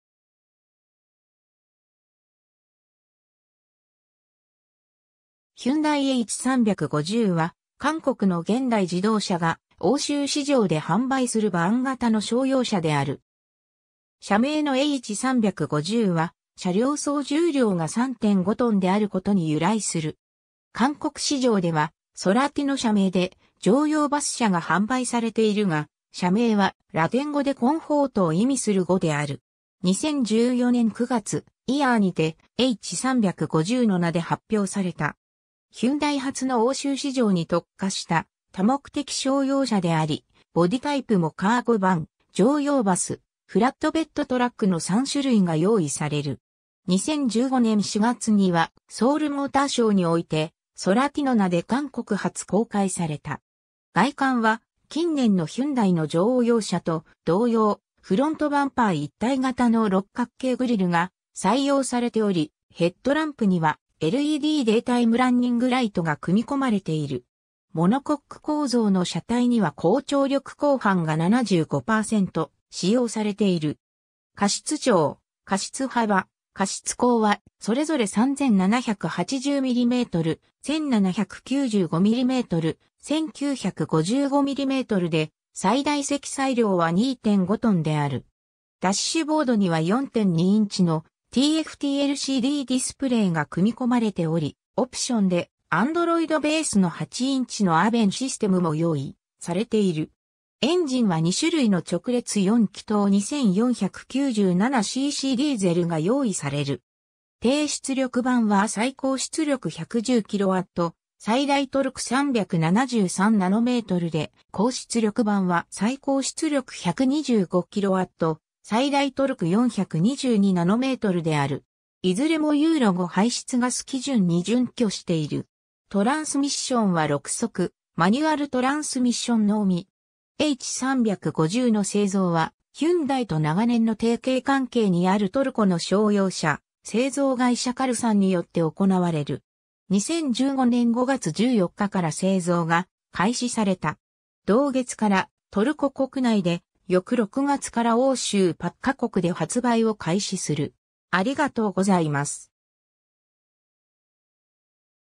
ヒュンダイ・H350
ヒュンダイ・H350 (Hyundai H350) は韓国の現代自動車が欧州市場で販売するバン型の商用車である。
車名のH350は車両総重量 (GVW) が3.5トンであることに由来する。
韓国市場では「ソラティ」（Solati, ハングル: 쏠라티、ソラチとも） の車名で乗用バス車が販売されているが、車名はラテン語でcomfortを意味する語である。
 2014年9月、IAA（ハノーファーモーターショー）にて「H350」の名で発表された。
ヒュンダイ初の欧州市場に特化した多目的商用車であり、ボディタイプもカーゴバン（GVW3.5トン、2/3人乗り）、乗用バス（GVW4トン、14/15/16人乗り）、フラットベッドトラックの3種類が用意される。
2015年4月にはソウルモーターショーにおいて「ソラティ」の名で韓国初公開された。
 外観は近年のヒュンダイの乗用車と同様、フロントバンパー一体型の六角形グリルが採用されており、ヘッドランプにはLEDデイタイムランニングライトが組み込まれている。
モノコック構造の車体には高張力鋼板が75%使用されている。
 荷室長、荷室幅、荷室高はそれぞれ3,780mm、1,795mm、1,955mmで、最大積載量は2.5トンである。
 ダッシュボードには4.2インチのTFT LCDディスプレイが組み込まれており、オプションでAndroidベースの8インチのAVN（オーディオ、ビデオ、ナビゲーション）システムも用意されている。
 エンジンは2種類の直列4気筒 2497cc ディーゼルが用意される。
低出力版は最高出力110kW (150ps)、最大トルク373Nmで、高出力版は最高出力125kW (170ps) 、最大トルク422Nmである。
いずれもEURO5排出ガス基準に準拠している。
トランスミッションは6速マニュアルトランスミッションのみ。
 H350の製造はヒュンダイと長年の提携関係にあるトルコの商用車製造会社カルサンによって行われる。
2015年5月14日から製造が開始された。
同月からトルコ国内で、翌6月から欧州8ヵ国で発売を開始する。